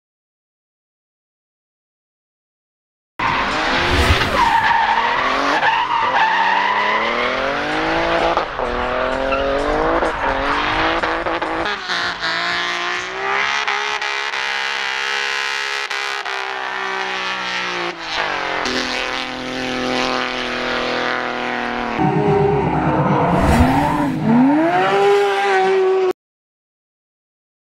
Drift.ro Shorts: Drifting in Croatia
Drifting on the airport of Osijek , in Croatia. JDM power followed by german goodness!